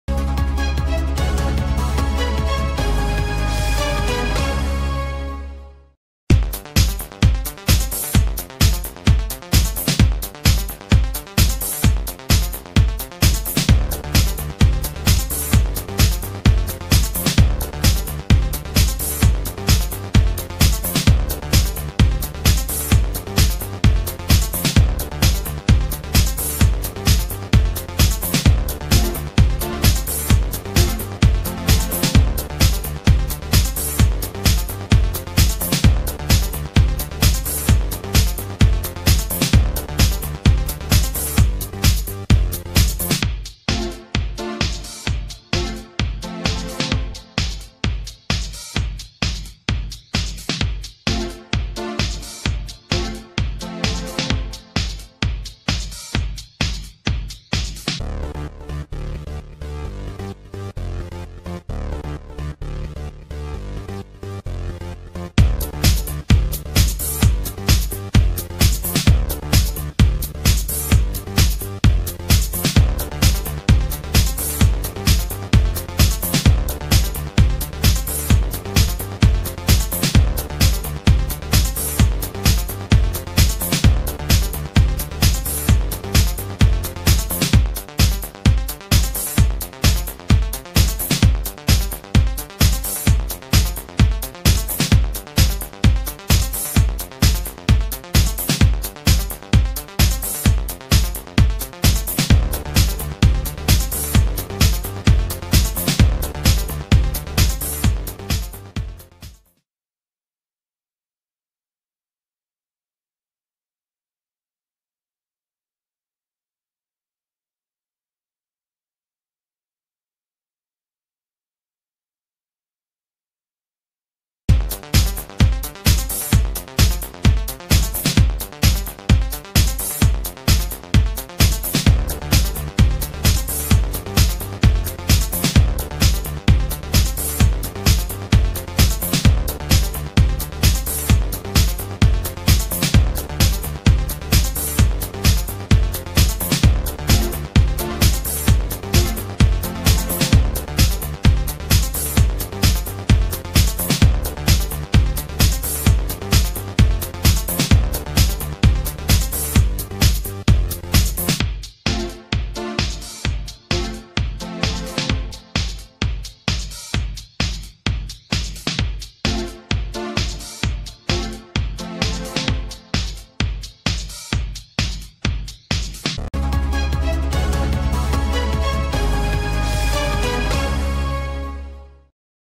Így tudta meg Rácz Jenő, hogy terhes a felesége: sejtik, hol fogant meg a baba
Rácz Jenő és Gyuricza Dóra júl...